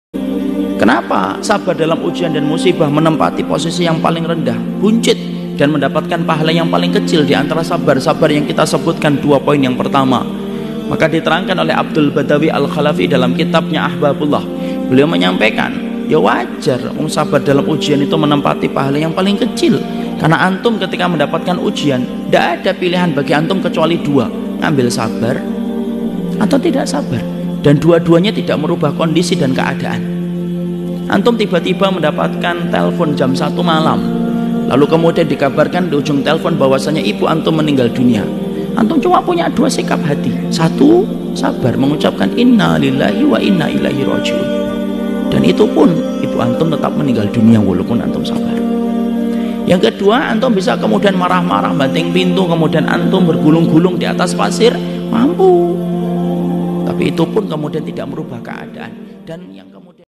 BERSABARLAH oleh Ustad Dr. Khalid Basalamah
"Kamu sungguh-sungguh akan diuji terhadap hartamu dan dirimu. Dan (juga) kamu benar-benar akan mendengar dari orang-orang yang diberi al-Kitab sebelum kamu dan dari orang-orang yang mempersekutukan Allah, gangguan yang banyak yang menyakitkan hati. Jika kamu bersabar dan bertakwa, maka sesungguhnya yang demikian itu termasuk urusan yang patut diutamakan" [Âli ‘Imrân/3 : 186]